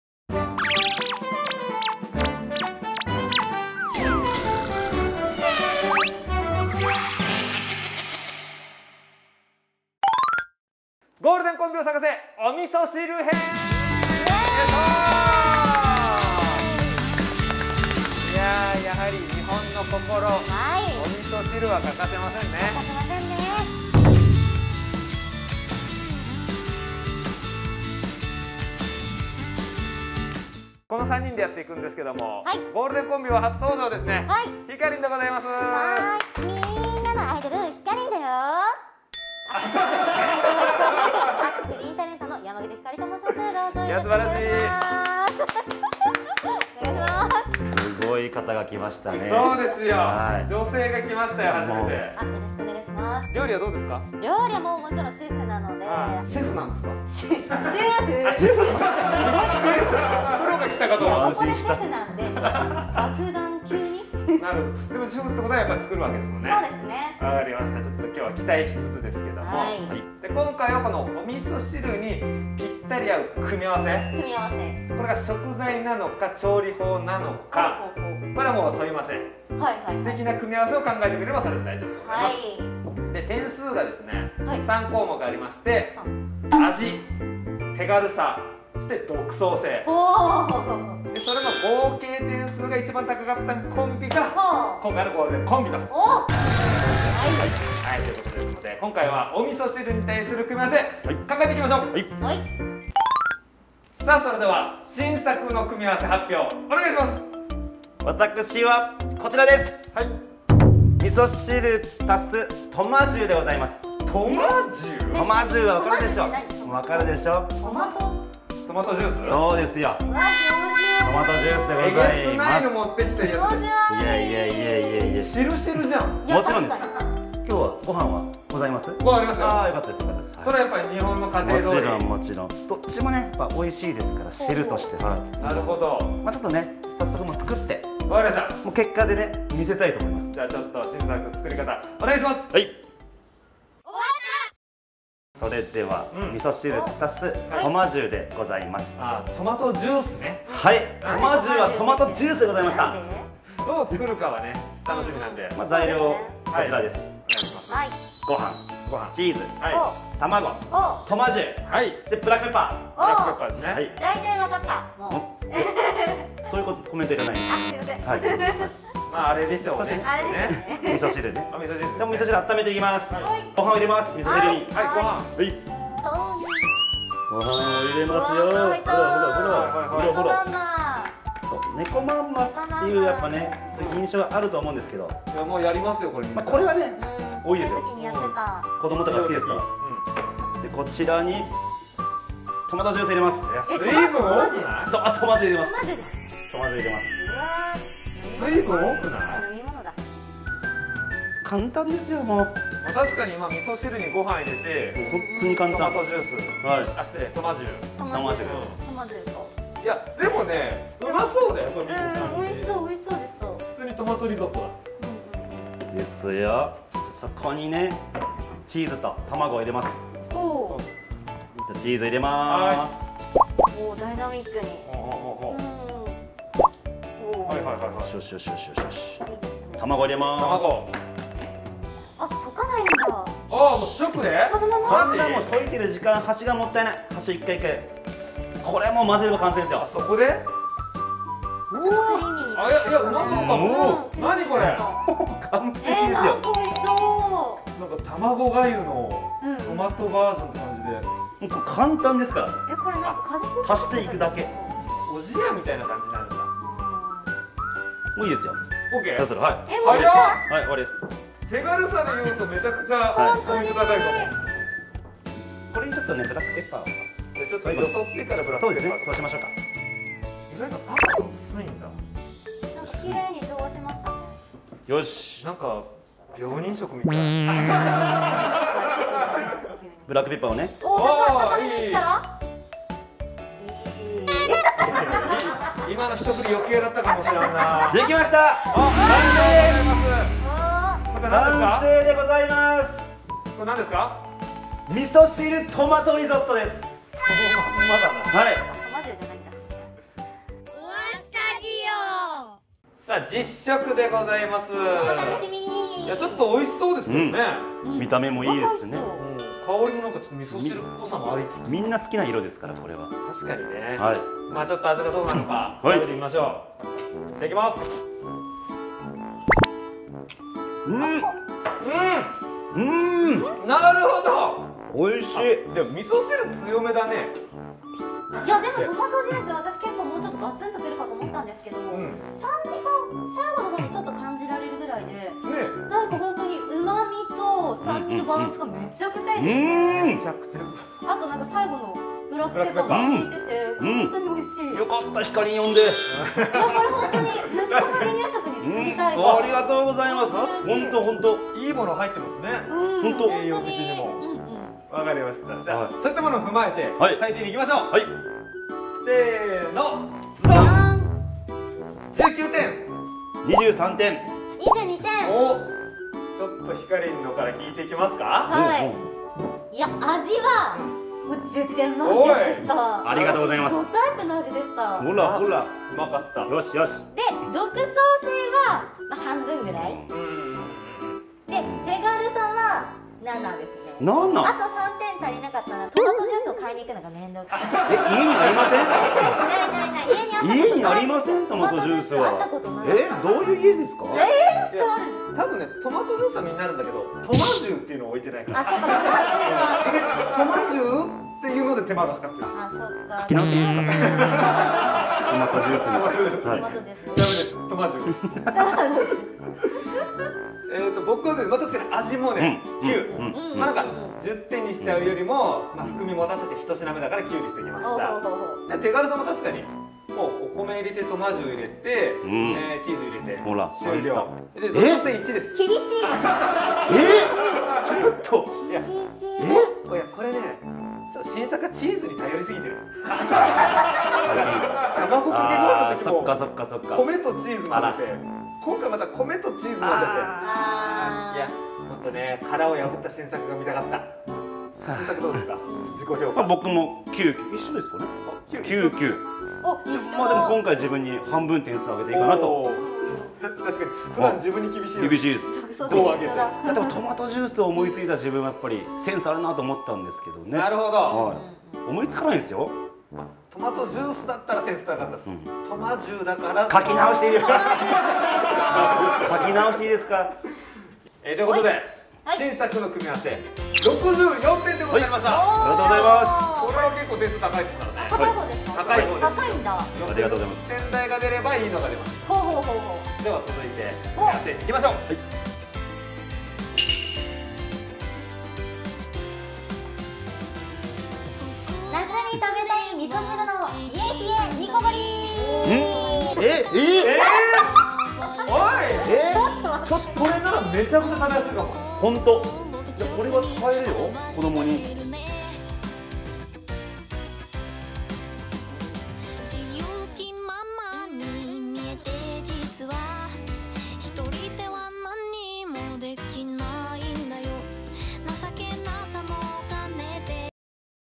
第7弾ゴールデンコンビを探せ（お味噌汁編）前編
郷土料理から創作料理まで、様々な料理に挑戦します。
今回はお味噌汁編
つばさ、ひかりん、新作、でお味噌汁に一番合う組み合わせを探してクッキンしマンデー！（新作編）

【材料】
・お味噌汁適量
・トマジュー雰囲気量
・ご飯一膳
・玉子一個
・チーズ適量
・ブラックペッパー少々

【作り方】
・温めたお味噌汁にご飯を投入
・トマジューを投入
・チーズを投入
・玉子を直で投入
・溶き混ぜたら盛り付けて最後にブラックペッパーをふる